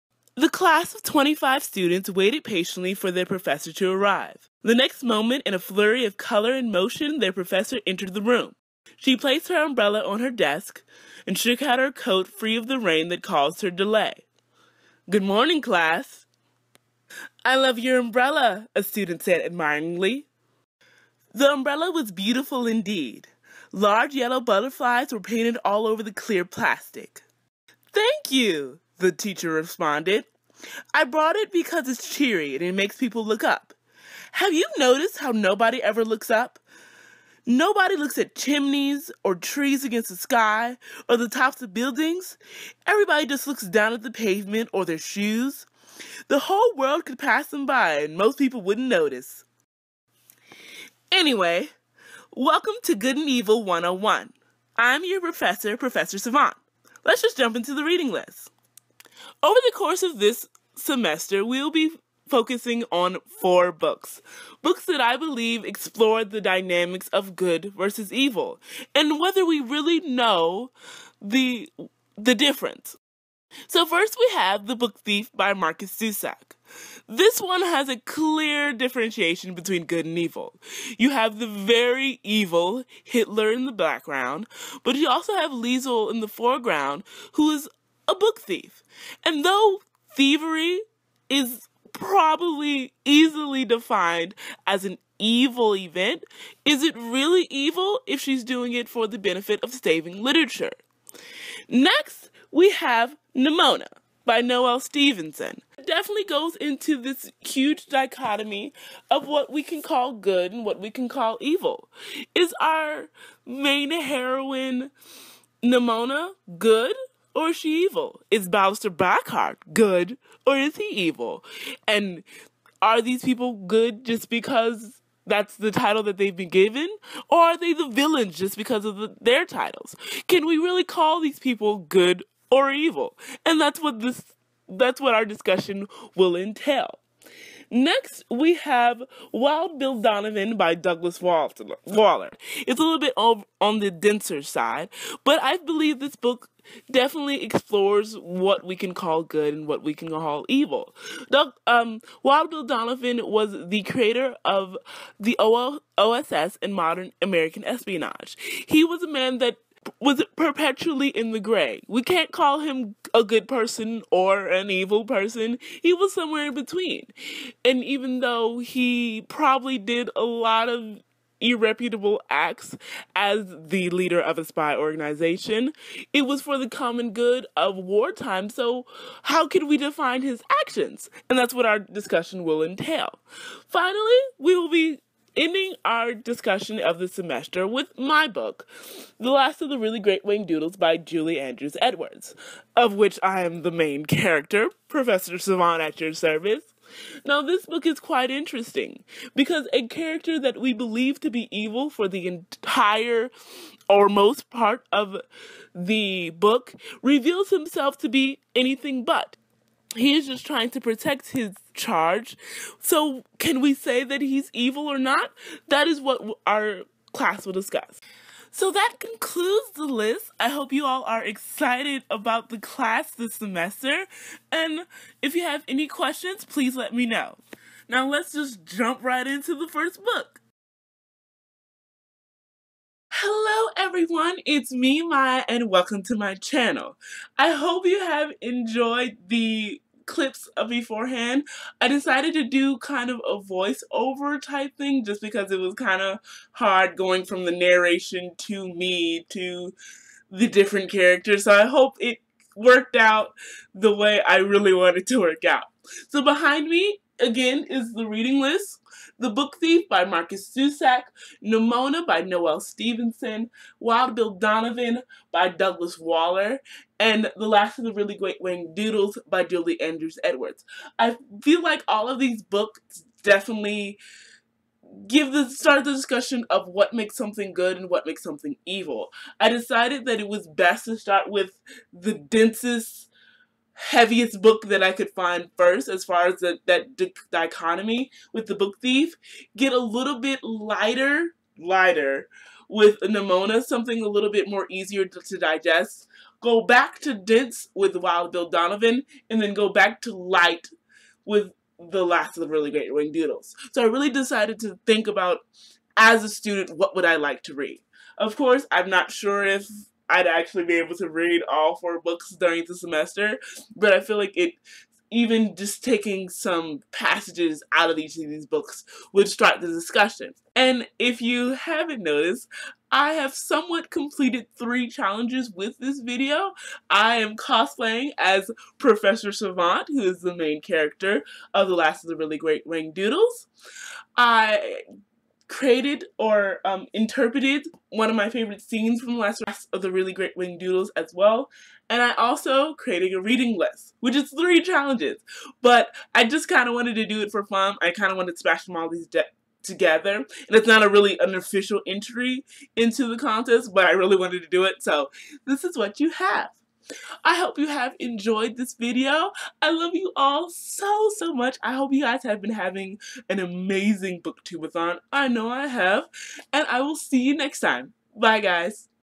Welcome to Good and Evil 101 || BookTube-A-Thon 2017
Welcome to Good and Evil 101 with your professor, Professor Savant from The Last of the Really Great Whangdoodles.

For this year's BookTube-A-Thon, I decided to challenge myself and combine some of the video challenges from the past week into one awesome video. I took the creativity a step further by making it primarily audio, so it feels like you're listening to an audiobook.

I post semi-regularly, so you'll definitely find me somewhere.